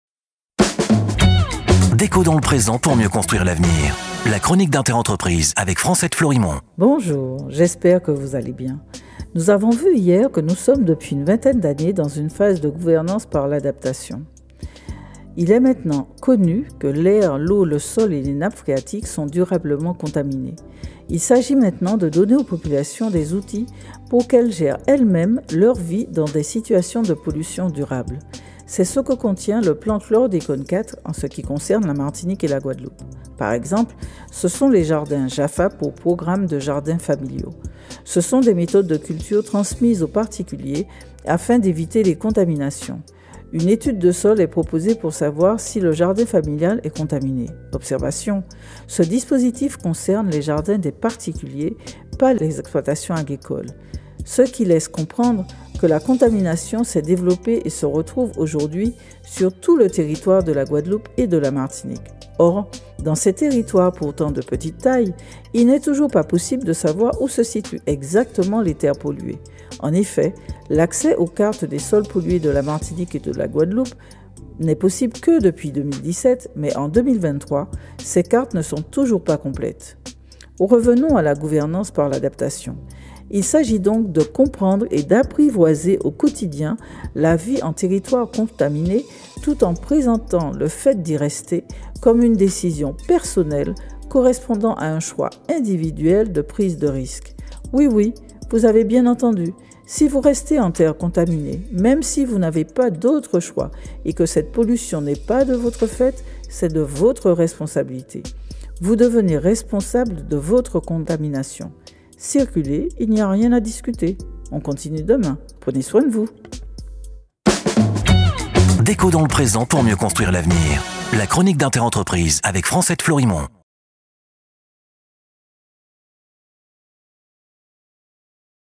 DLPPMCA • Contamination aux pesticides - Partie 17
Les populations responsables de leur contamination !